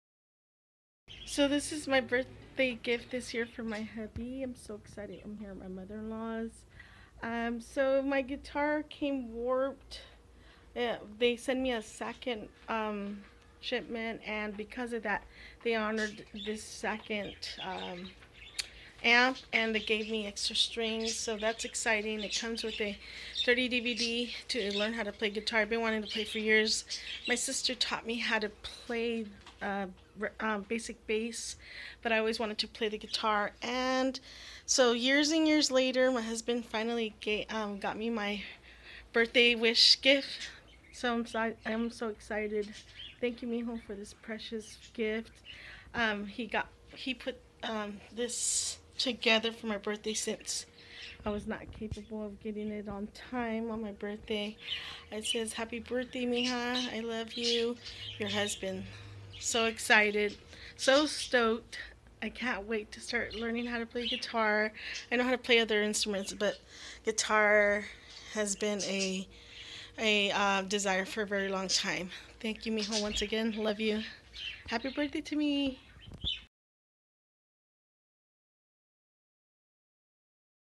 LMT TV My Bday Wish Gift My Boo Gave My So Stoked :)
Thank you babe from my Bday gift I've been wanting for so long love you Boo :)